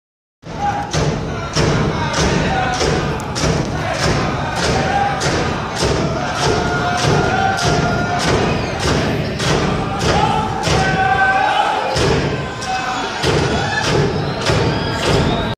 Native Alaskan Dance and song (loud)
Alaska Federation of Natives 2018 - A short clip of one of the Alaska Native dance groups that were invited to the convention that year. Anchorage, Alaska.